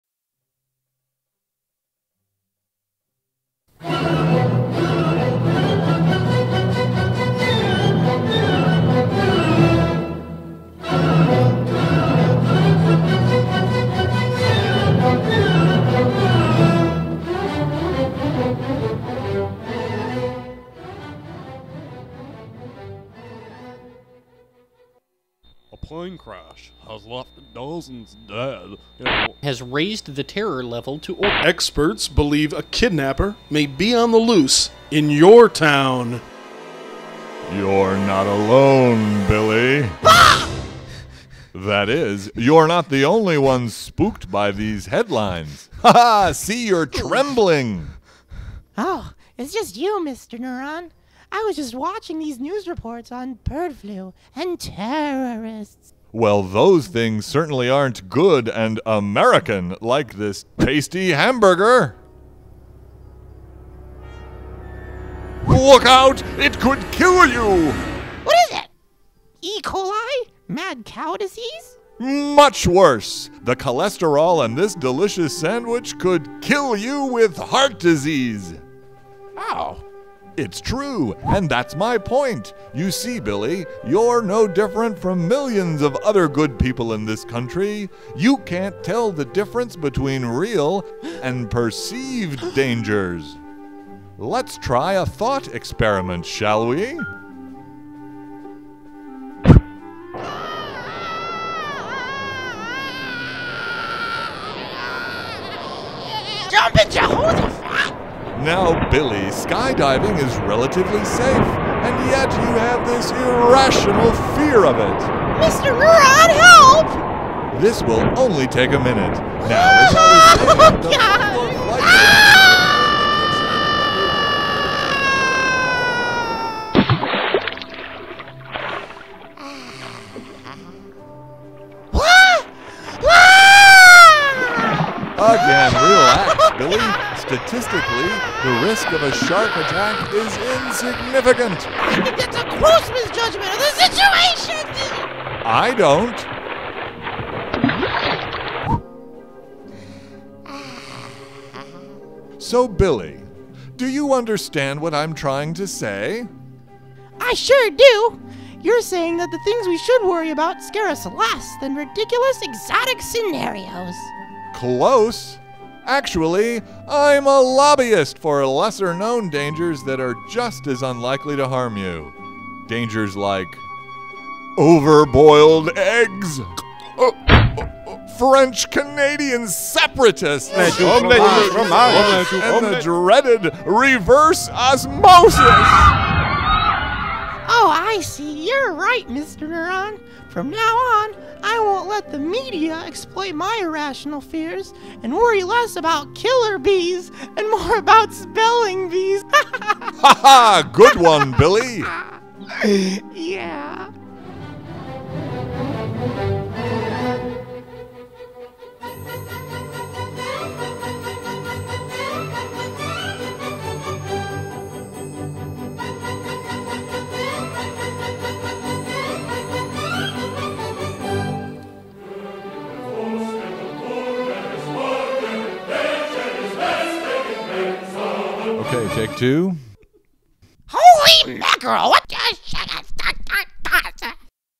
The Edutastic Adventures of Mr. Neuron
Mr. Neuron and Billy the Hesitant child explore the heated issue of fear-mongering and scare tactics.